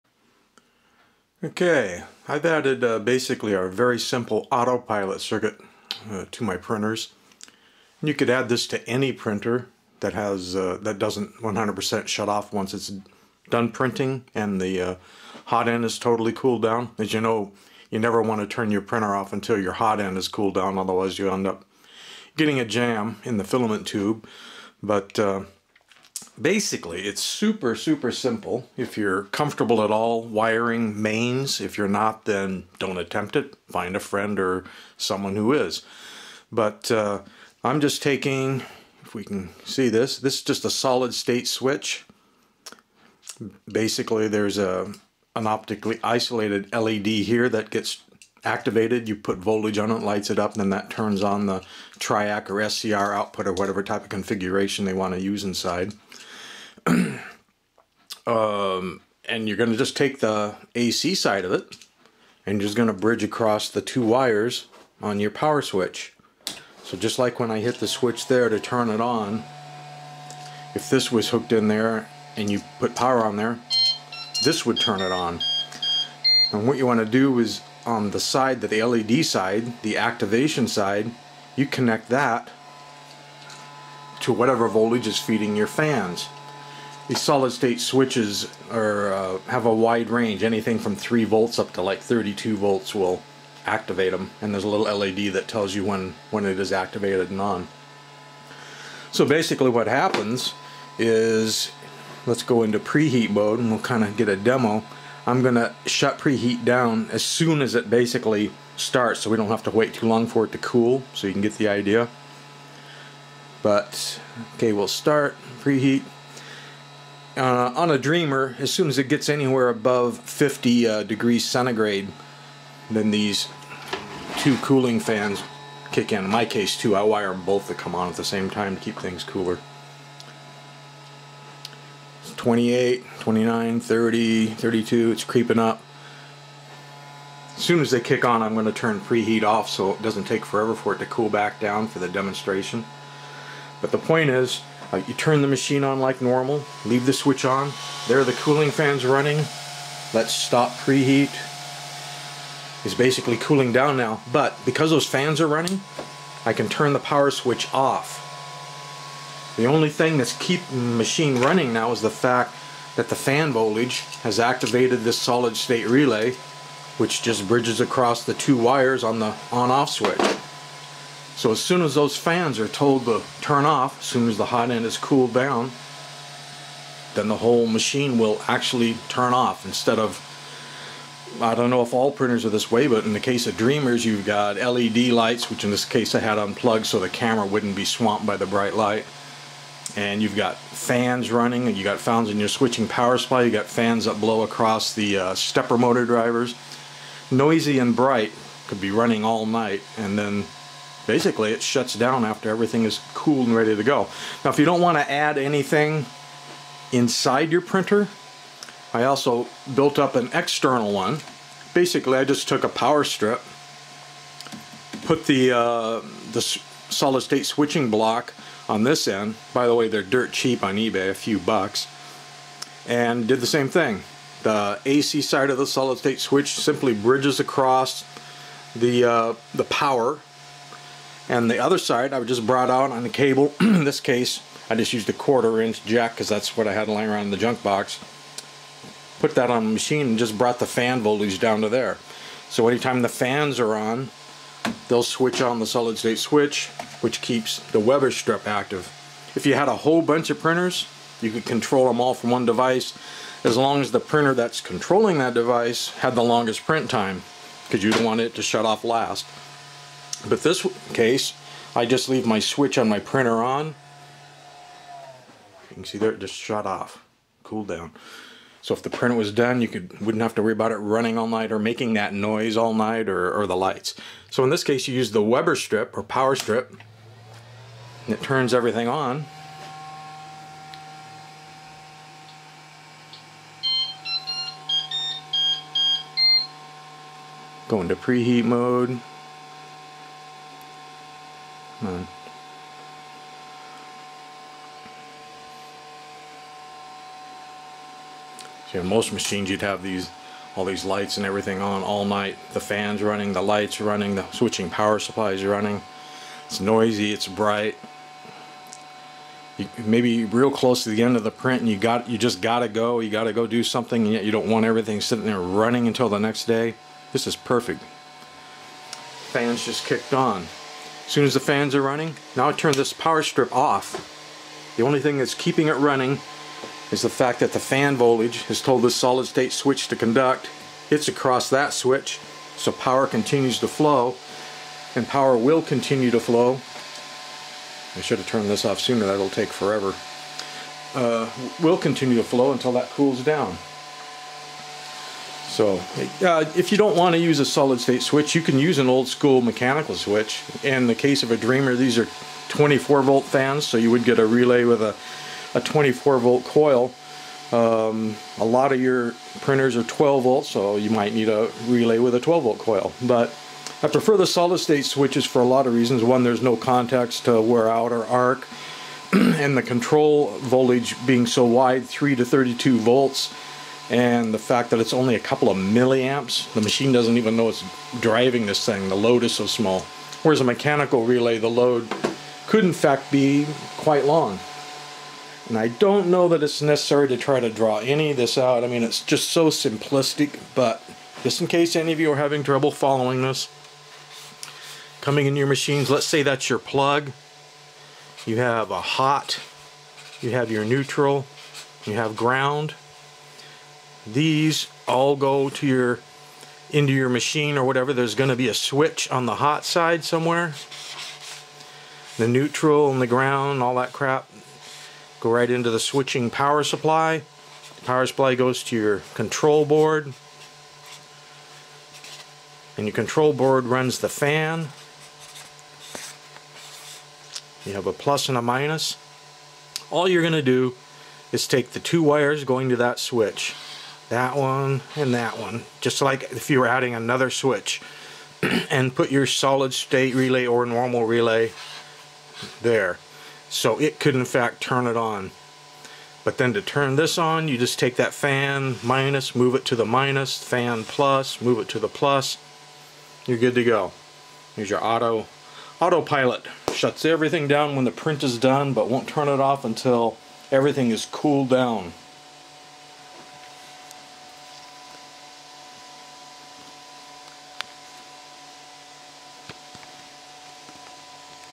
Add AutoPilot to your 3d printer. 100% shut down after print / Hotend cool down.
All printers are different but many have lights and fans and a power supply that stay on even after the print is done. By adding a simple SSR "solid state relay" or a old school mechanical relay, you can have your printer turn OFF when the print is done and the hot end has cooled down. This can be placed in the printer or made a external device. The relay switching device is wired across the power switch so that it can turn ON the power or the switch can turn on the power. The control for the relay device is powered from the Hot end cooling fans. To operate you turn ON your printer like normal and start a print. As soon as the hot end fans are running you can now turn OFF the printer power switch. The printer will stay on since the relay device is energized by the Hot end cooling fans. Once the print is done and the Hot end is cool the fans turn off and so does the relay device. You can make this a external unit by moding a power strip. You will need to bring out the fan power wires to control the relay but that's all. Since this does involve wiring Mains voltage devices you should not do any of this unless you know what your doing. But if you have ever changed a light switch or AC wall plug then you have already done something much more involved.